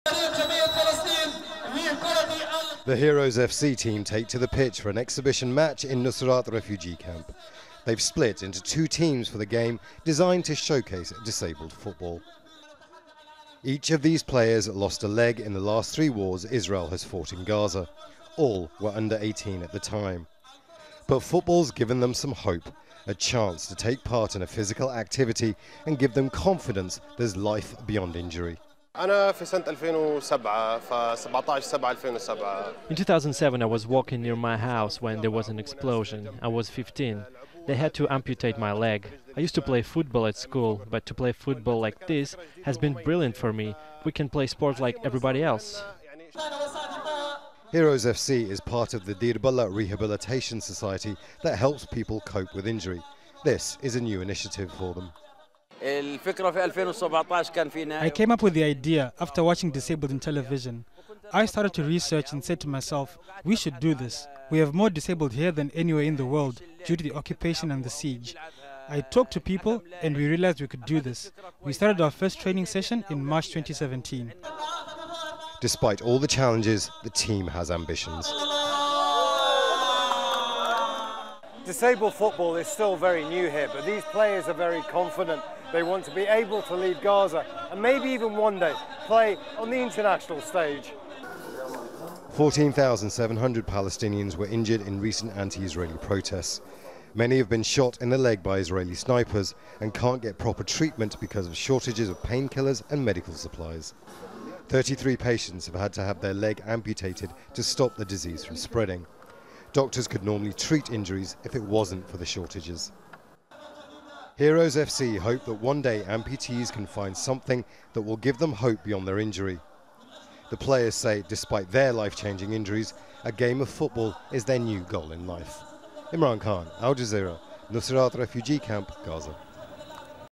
🇵🇸 Playing football brings hope to amputees in Gaza | Al Jazeera English
As international football teams compete to qualify for the World Cup in Russia, a rehabilitation centre is helping some amputees in the occupied Palestinian territory Gaza play sport like everyone else. Hundreds of Palestinians lost their legs in wars and protests against Israel, but the initiative is helping the disabled live a more normal life and get back into the game.

Al Jazeera's Imran khan reports from Gaza.